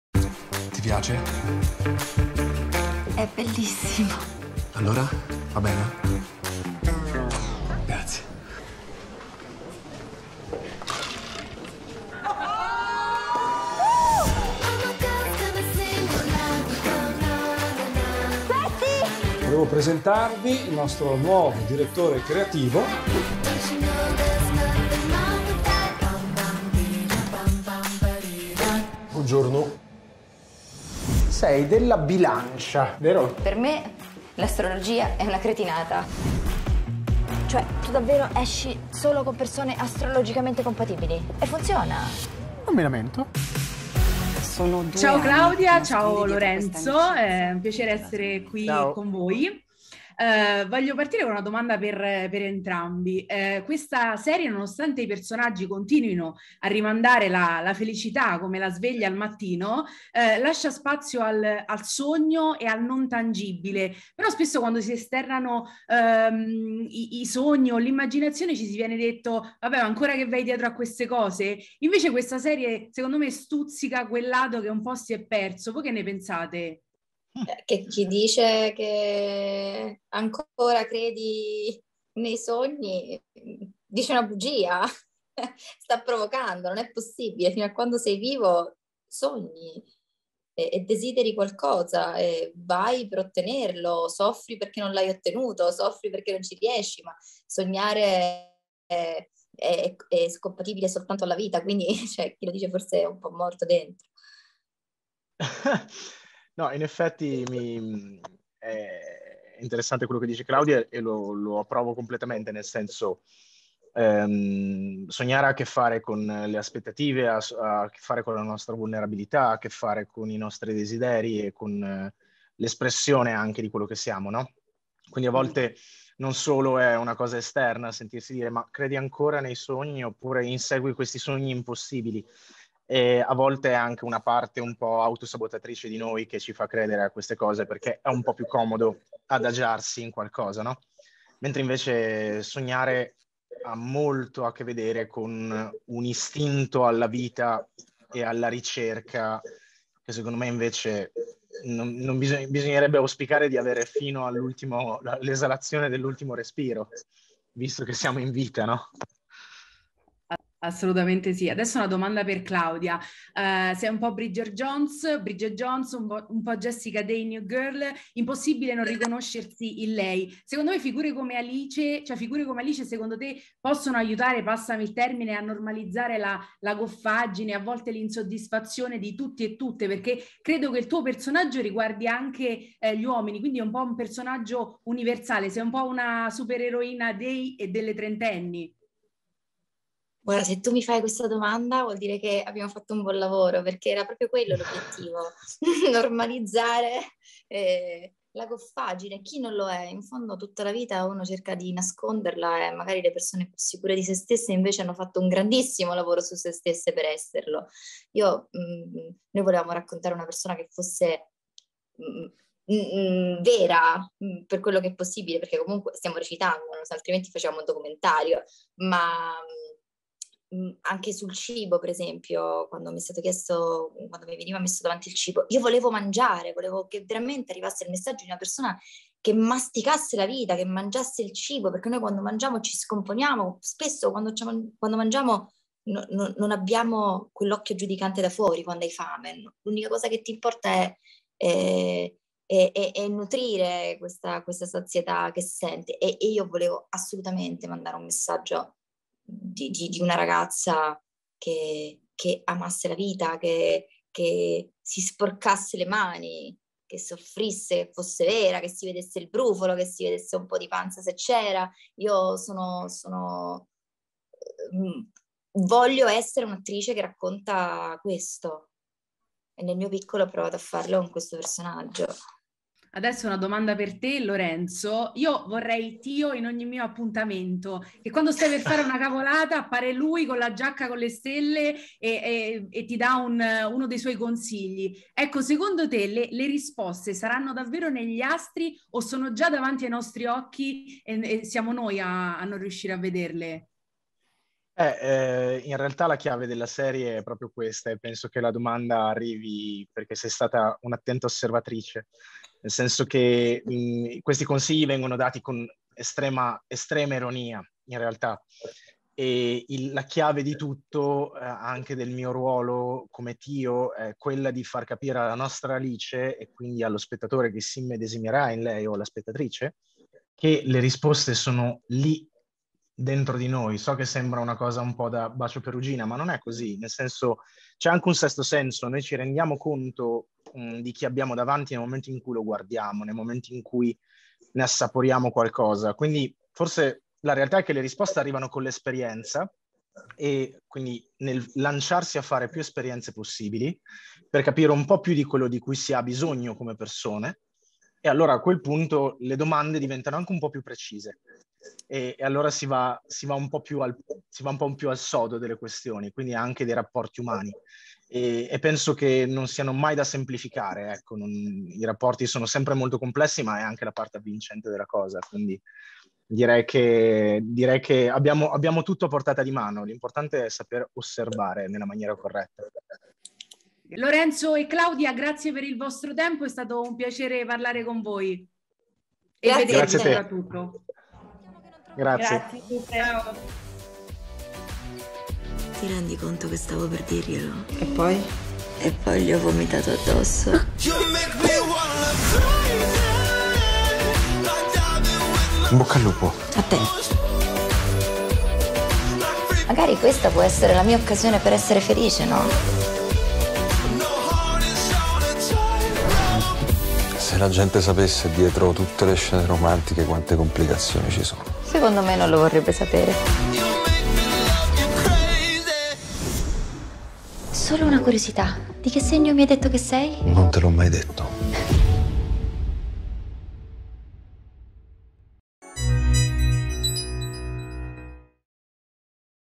Guida astrologica per cuori infranti, l'intervista a Claudia Gusmano e Lorenzo Adorni
Problemi d'amore? Problemi nella vita in generale? La soluzione è nelle stelle o è davanti ai nostri occhi e siamo noi a non accorgercene? A metterci davanti a quest'ultima domanda è 'Guida astrologica per cuori infranti': la nuova serie italiana Netflix, in 6 episodi, creata da Bindu de Stoppani e diretta dalla stessa Bindu e da Michela Andreozzi. Prodotta da Italian International Film - Gruppo Lucisano e tratta dall’omonimo successo editoriale di Silvia Zucca edito da Casa Editrice Nord, la serie debutta oggi sulla piattaforma e in tutti i Paesi in cui il servizio è attivo.